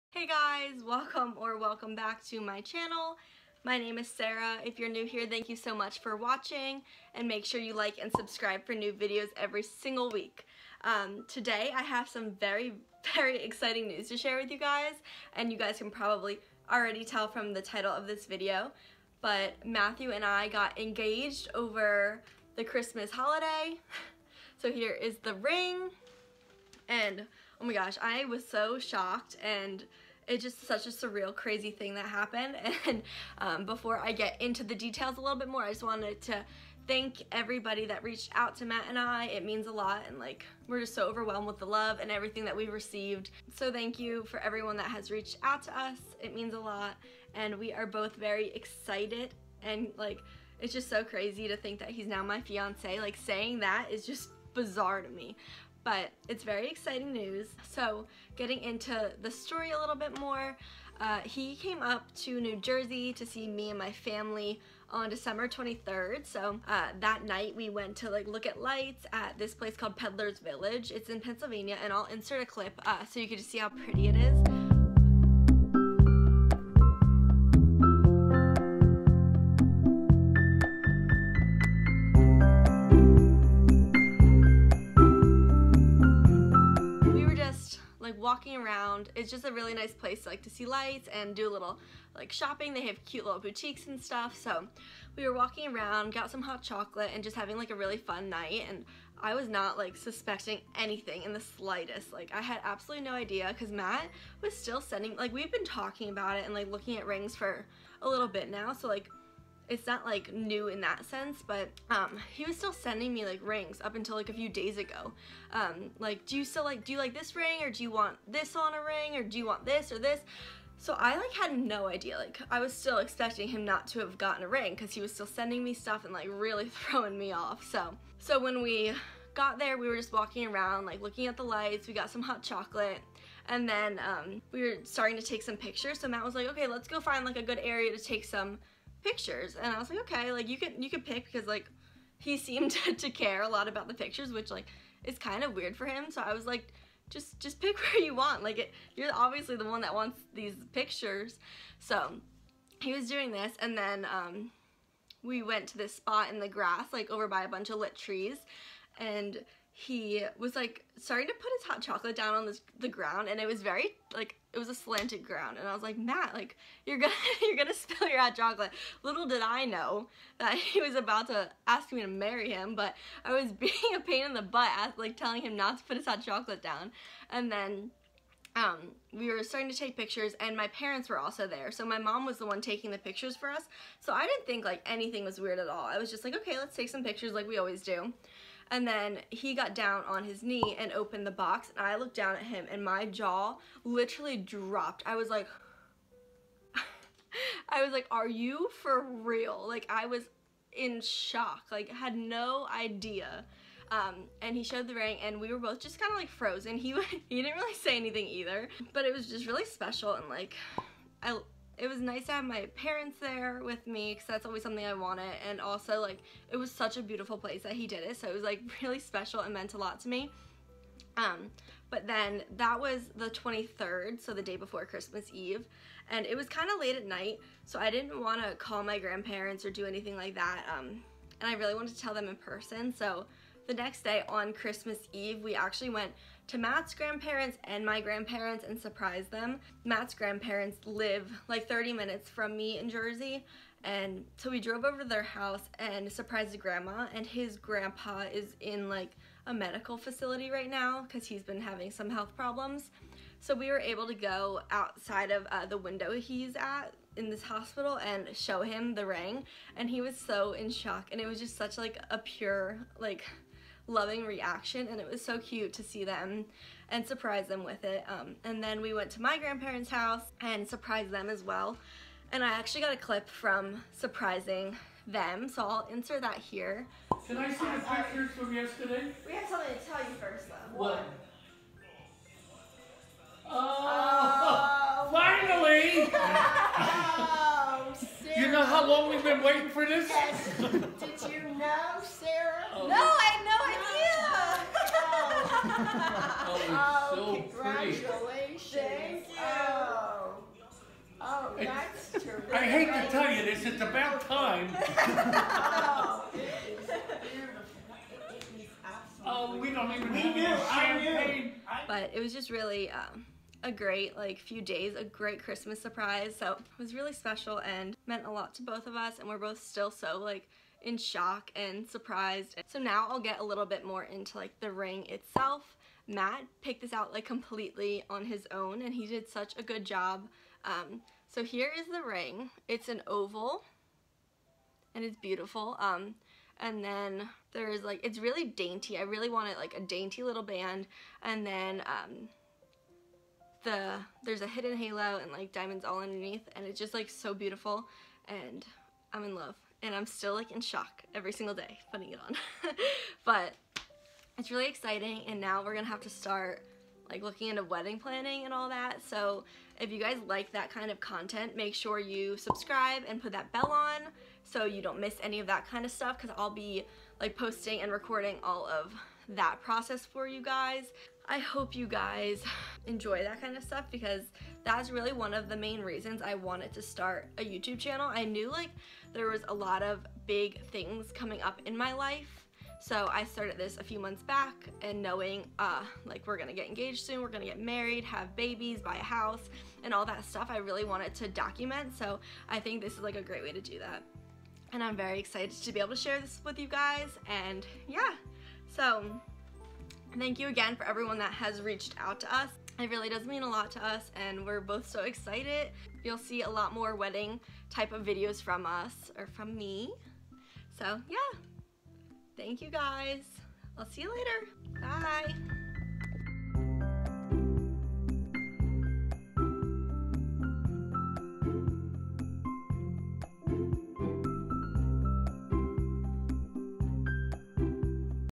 WE'RE ENGAGED!! | CHRISTMAS 2020 PROPOSAL VIDEO
I'M ENGAGED!!! I am still in shock that I am going to marry my best friend! Matt and I are so excited to start this next journey together!!

This video goes in depth of the proposal and the engagement ring! I wish we had a video of the proposal to share with you all but the pictures will have to do!! :)

Thank you to everyone that has reached out and congratulated us!! We are overwhelmed with the love we received!!

Sarah's IG: @sarahpopolo
Matt's IG: @mattchrist65
Frank's IG: @frankiethecavachon